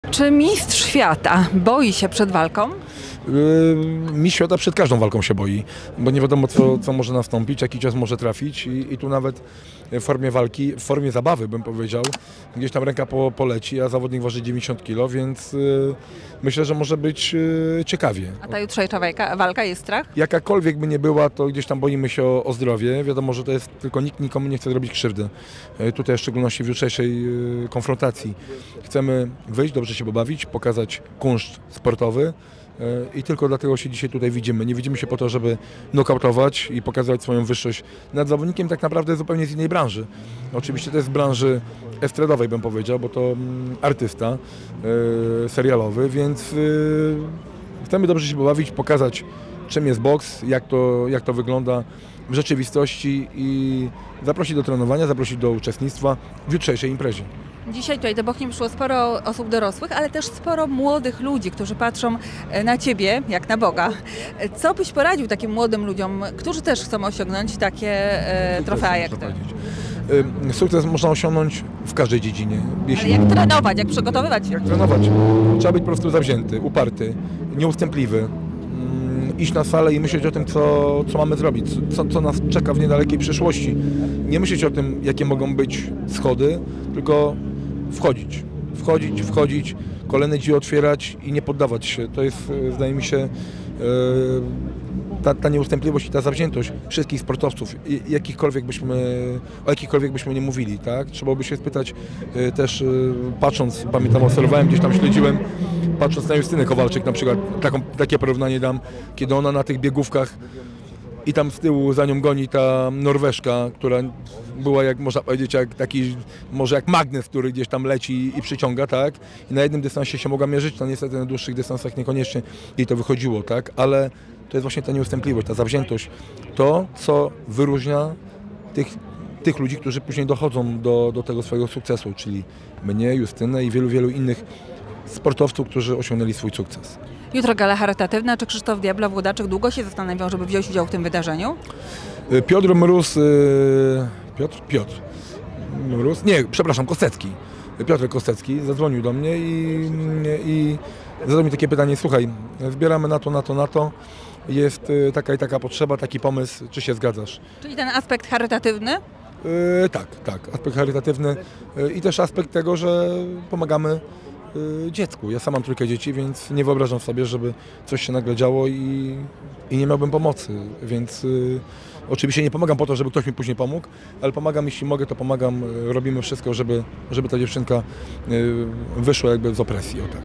Krzysztof "Diablo" Włodarczyk: czuję strach przed każdą walką
Luksusowymi samochodami przyjechali na bocheński Rynek zawodnicy zaplanowanej na sobotę Gali Prime Boxing Night Pełna Moc. W samym centrum miasta w piątkowy wieczór odbyła się widowiskowa ceremonia ważenia i konferencja prasowa. Było też mnóstwo innych atrakcji.

W Bochni pojawili się: Krzysztof „Diablo” Włodarczyk, Artur „Kornik” Sowiński, Rafał Jackiewicz, Piotr „Bubu” Ćwik, Piotr Boguciński, aktor Piotr Mróz oraz liczni reprezentanci regionu bocheńskiego.

O to, czy mistrz czuje strach przed walką  - pytaliśmy  mistrza świata organizacji WBC oraz byłego mistrza świata organizacji IBF w kategorii junior wagi ciężkiej. W sobotę Krzysztof Włodarczyk zmierzy się z Piotrem Mrozem.